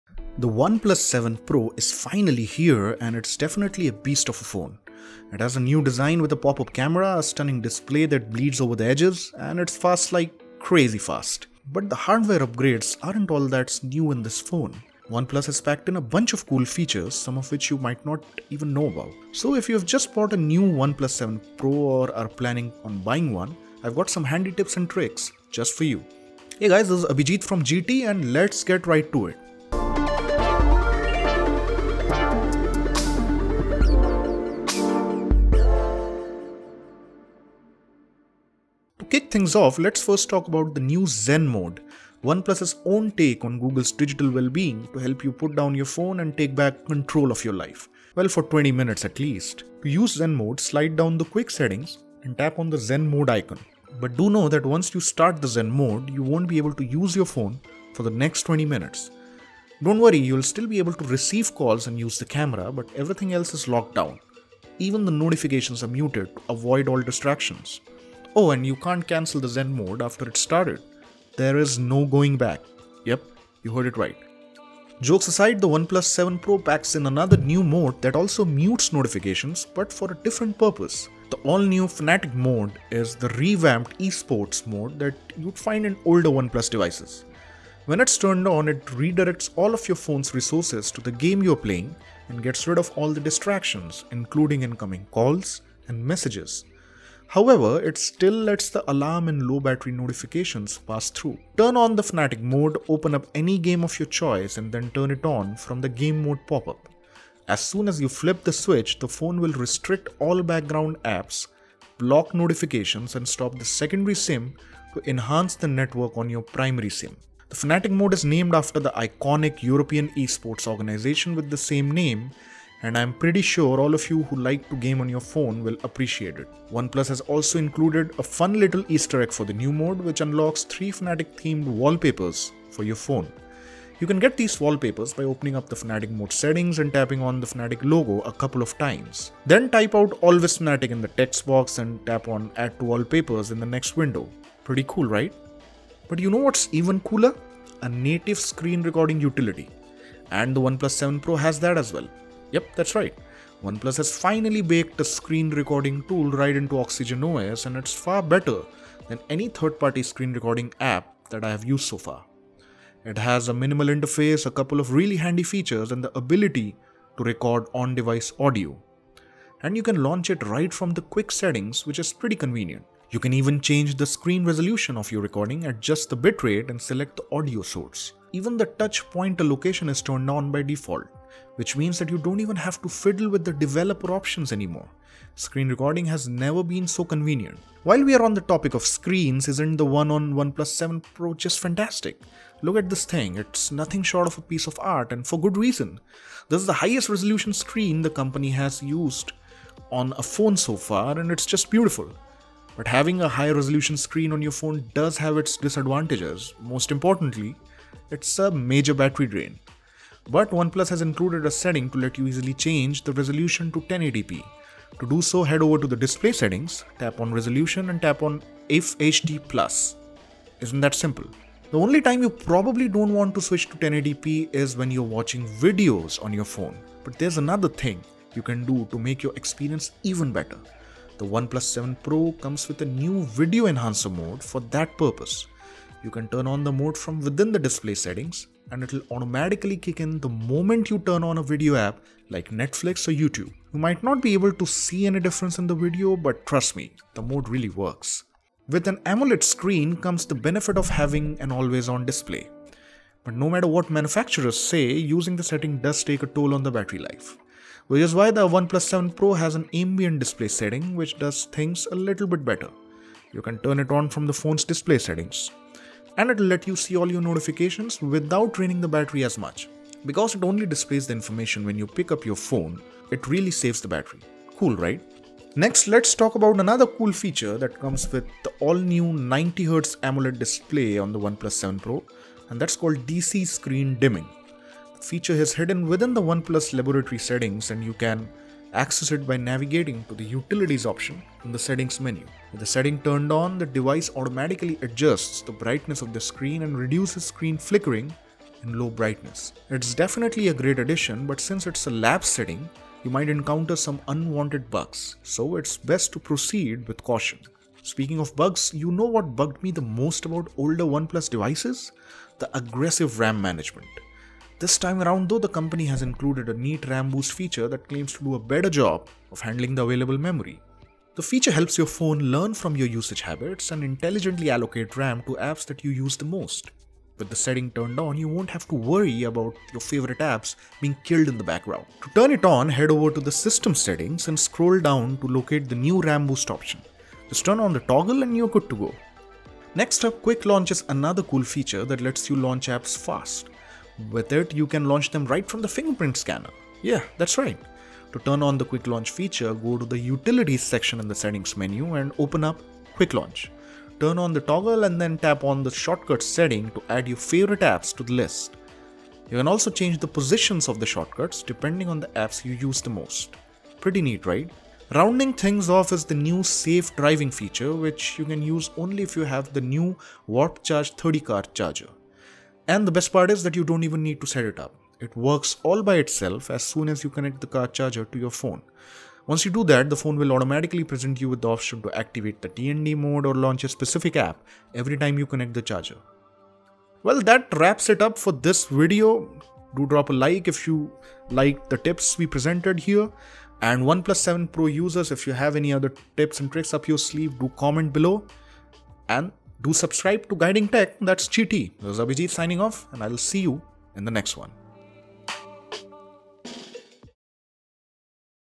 OnePlus 7 Pro - 10 AWESOME TRICKS YOU MUST KNOW! | Guiding Tech
Here are the best tips and tricks of OnePlus 7 Pro that every user must know. Some of these OnePlus tricks are hidden deep under the Oxygen OS 9.5 settings and are hard to locate.

So just to make sure you don't miss out on these settings to make the most of the camera, software, and the phone overall hardware.

Don't forget to tell us which if your favorite OnePlus 7 Pro trick of all and tip our users.